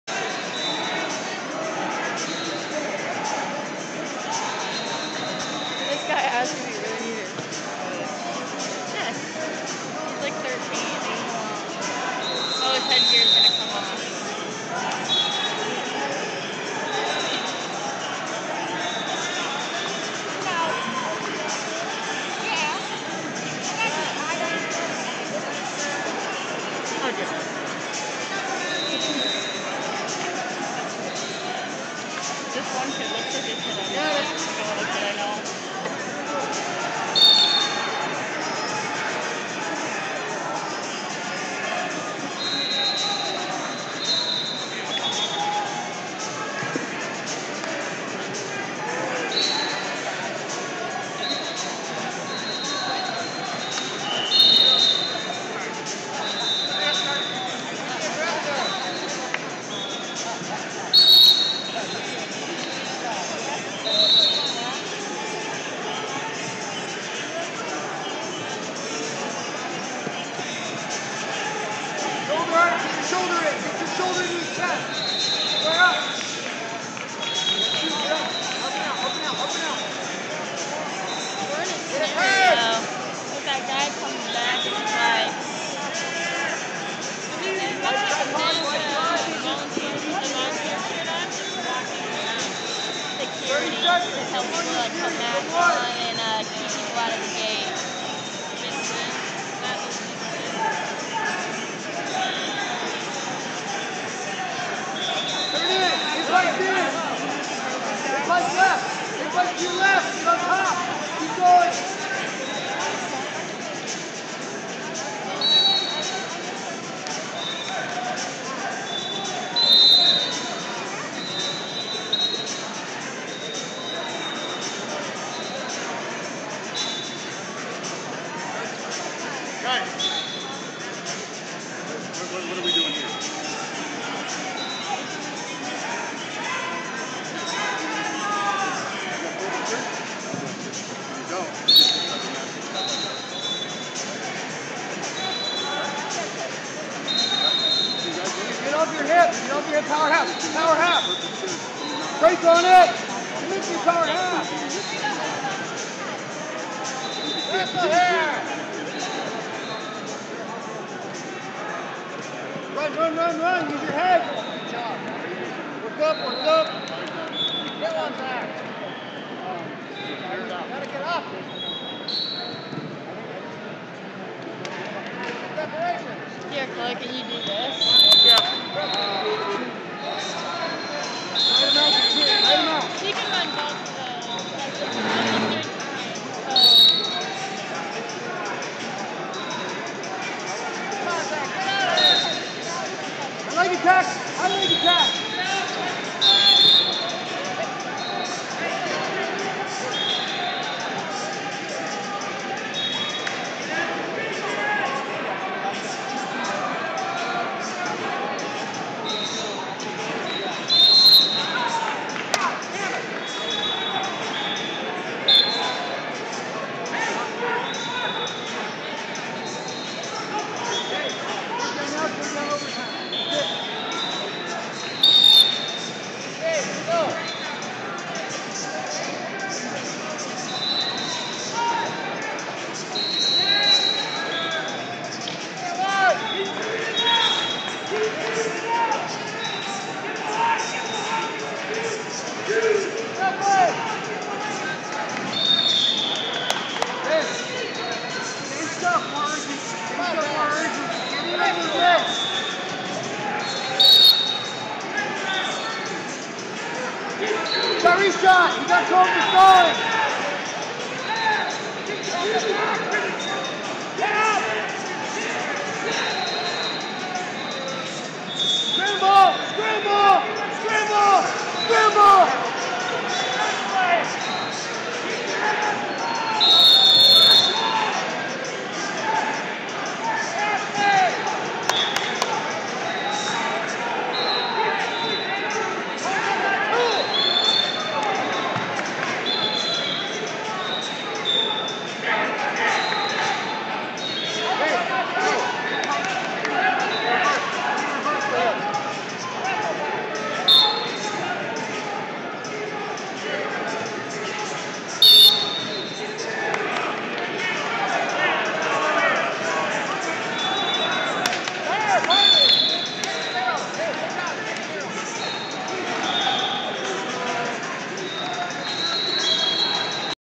2020 USAW Preseason Nationals/2044 High School 11th-12th Grade 120 Estevan Encinias 505 Wrestling
2020 USAW Preseason Nationals - 2020 USAW Preseason Nationals/2044 High School 11th-12th Grade 120 Estevan Encinias 505 Wrestling Club Vs Zach Tracy Scott West